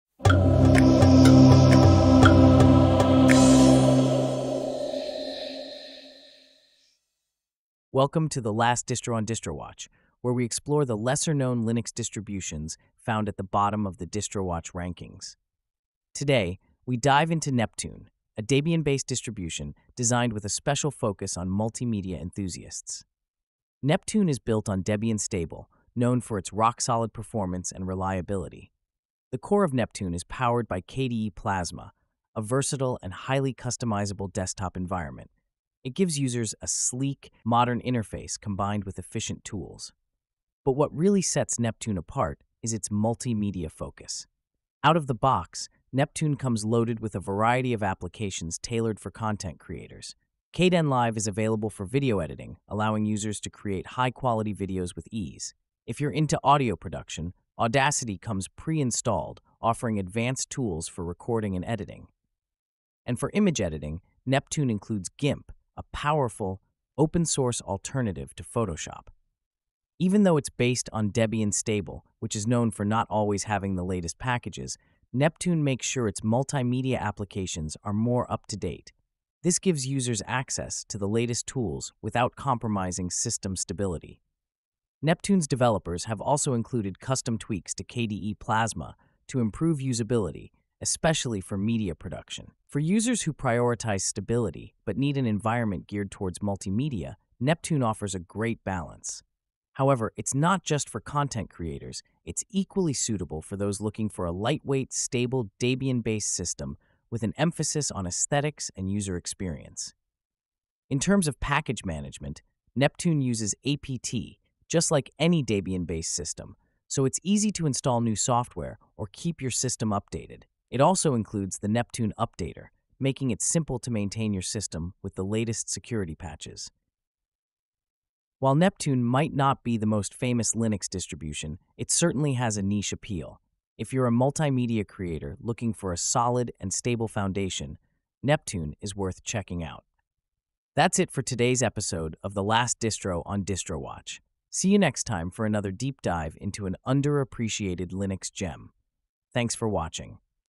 Neptune OS - The last Distro on Distrowatch
In this episode of "The Last Distro on Distrowatch," we explore Neptune OS, a Debian-based Linux distribution tailored for multimedia enthusiasts. Discover the unique features Neptune offers, including a customized KDE Plasma desktop, pre-installed multimedia tools like Kdenlive, Audacity, and GIMP, and its balance between stability and up-to-date software. If you're a content creator seeking a stable Linux experience with advanced media capabilities, Neptune OS is worth a closer look!